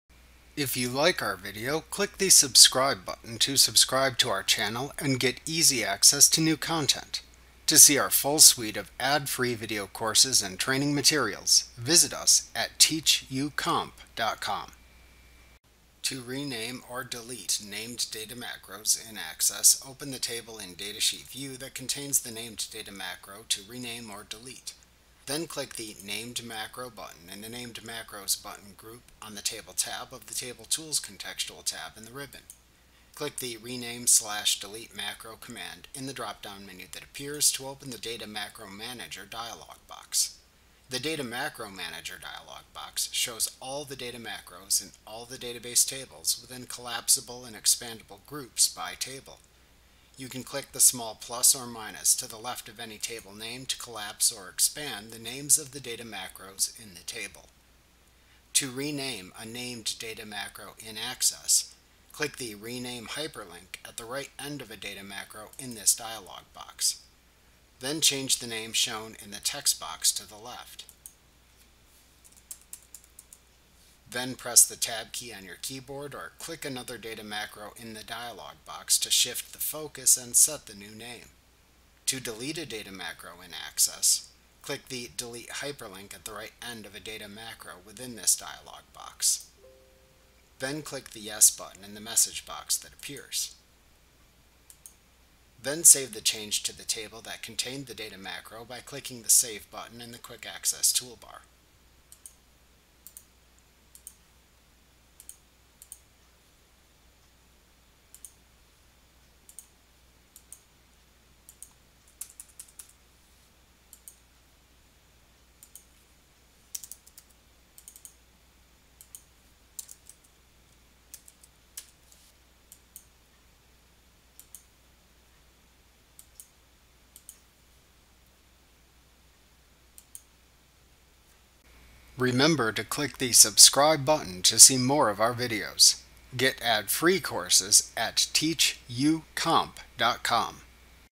Access 2019 & 365 Tutorial Renaming and Deleting Named Data Macros Microsoft Training
Learn about Renaming and Deleting Named Data Macros in Microsoft Access 2019 & 365 with the complete ad-free training course